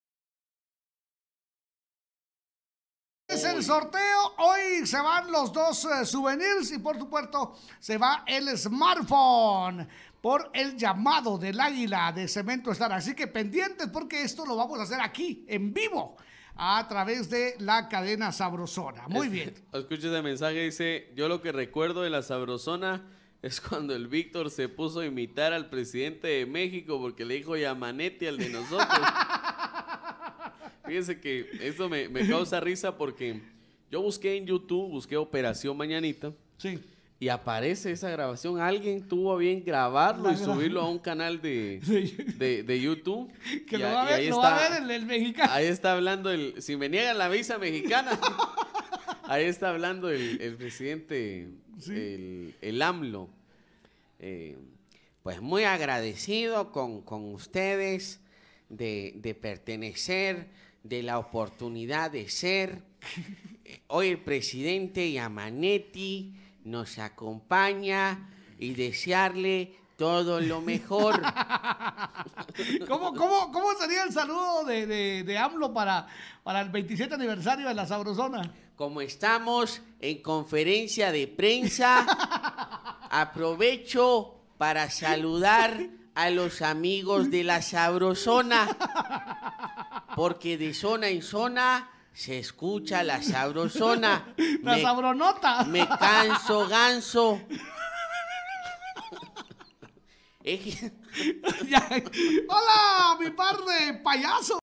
Saludos de "AMLO" a La Sabrosona por sus 27 años.
Aprovechando la mención indirecta que Victor García (izq) me hizo, por subir esa grabación donde imitó al presidente mexicano, les mando saludos yo también por su aniversario #27.

Rec: 26/08/22